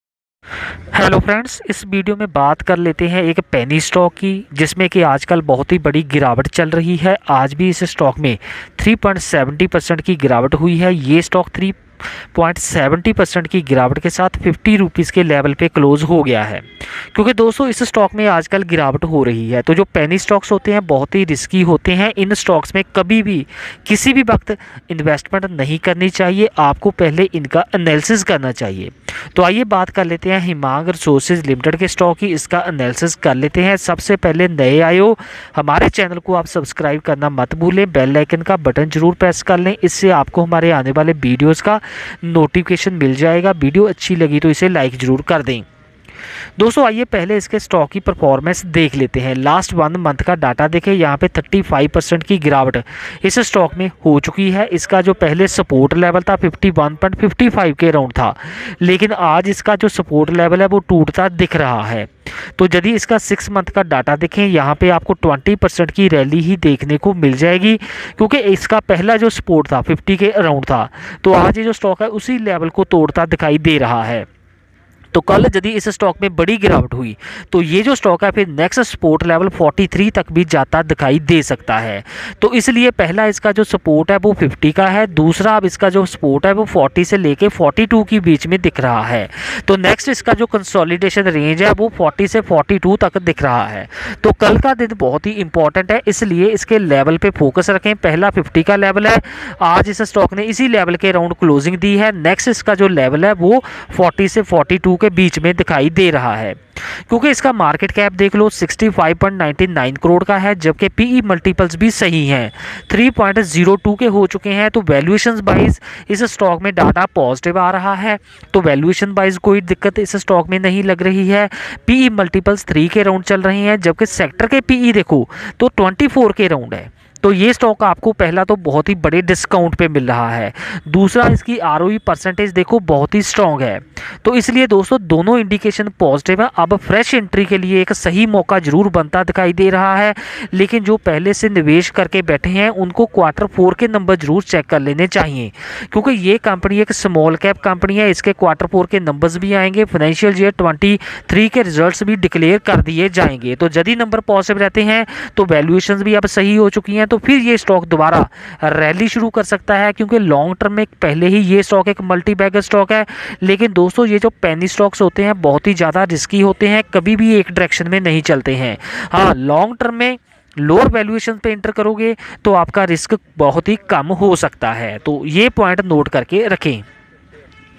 इस penny शेयर में निवेश की सलाह, अब इन levels पर focus करें, Fresh entry के लिए SIP करते रहो//
हेमंग रिसोर्सस शेयर analysis

Other topics:
Stock investment
Stock analysis
Stock recommendations